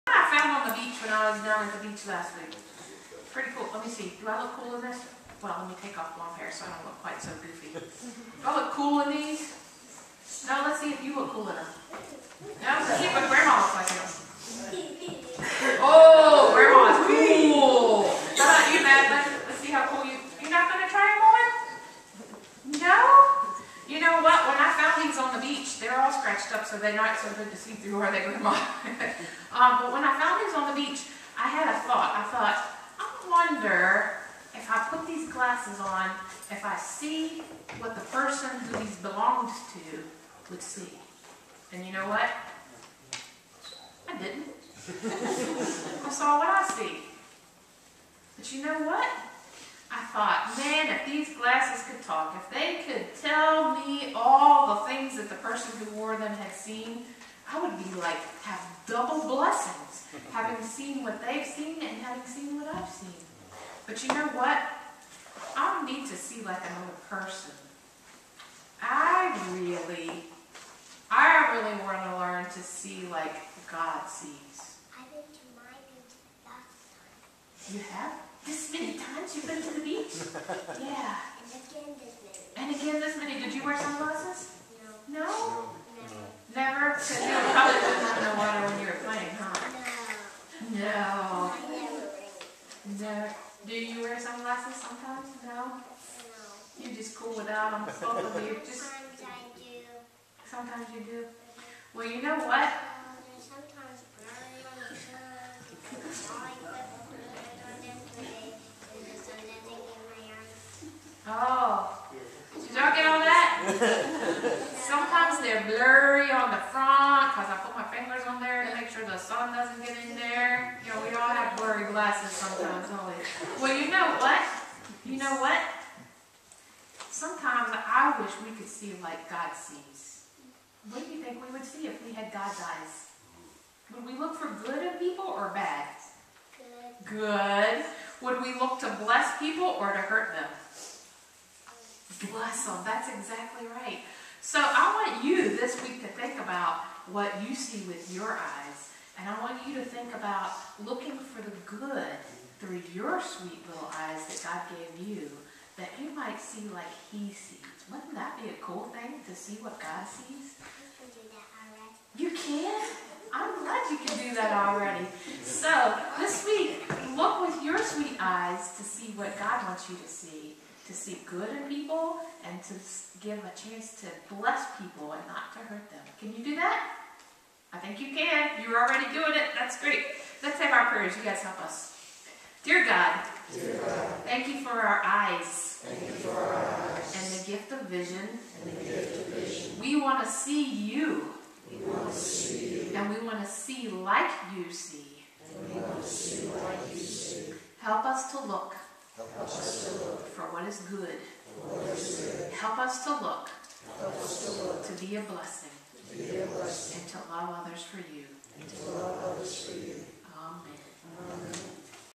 Looking Through God's Eyes
Children's Message by Michelle Edmonds Lindsey at Wesley Chapel United Methodist Church on October 25, 2015.